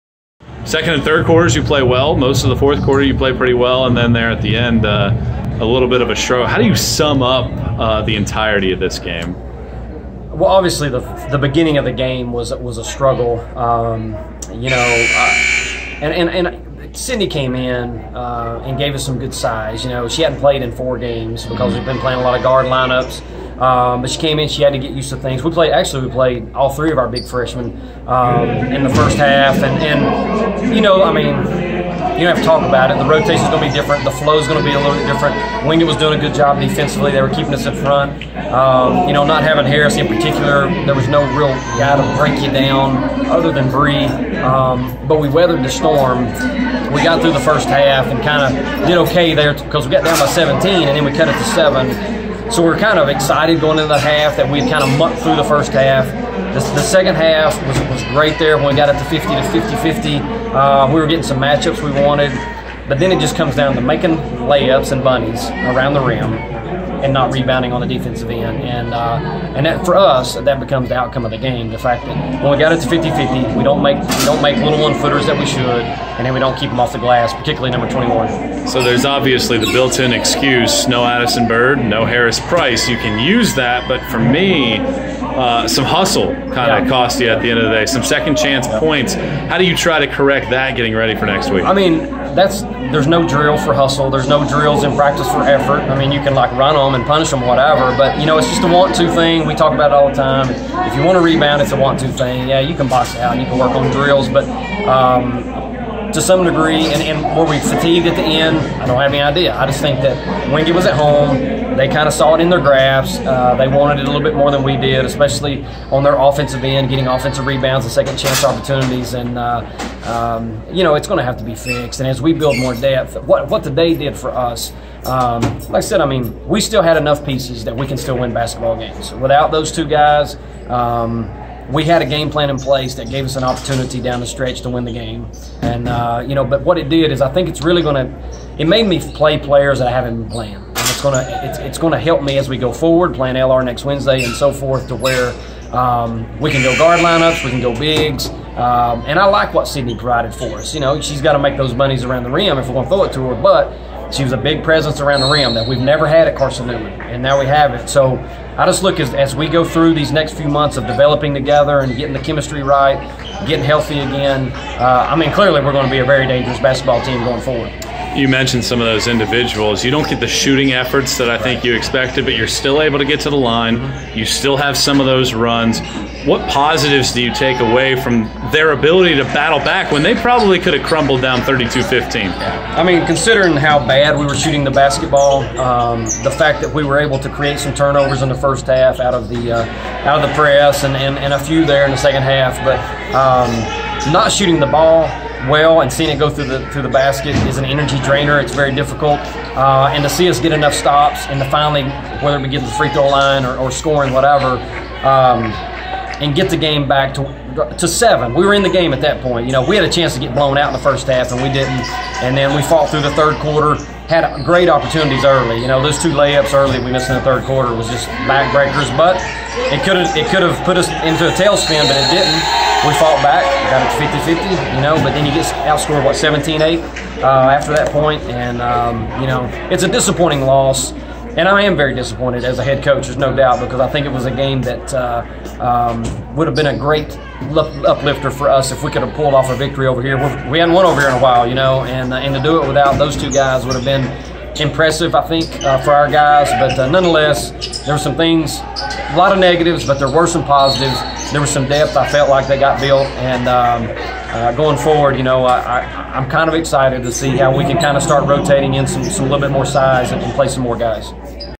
Carson-Newman Women's Basketball: Mike Mincey Recaps Wingate 12-1-18
Down a pair of starters averaging 14 points each, the Lady Eagles were unable to close in their second loss in three games.